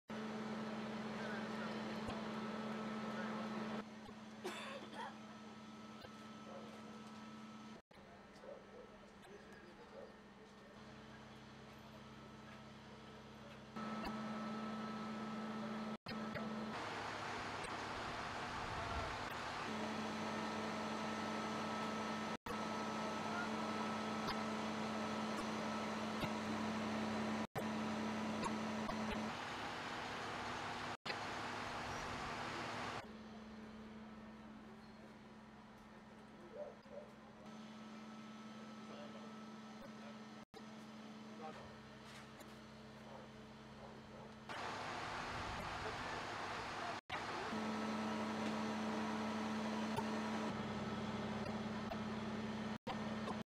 Small plane crashes into SE Austin home
A small plane crashed into the backyard of a home northwest of Austin-Bergstrom International Airport.

The plane went down at approximately 1:15 p.m. off Suena Drive, which is west of Highway 183 and north of Riverside Drive in the Montropolis area.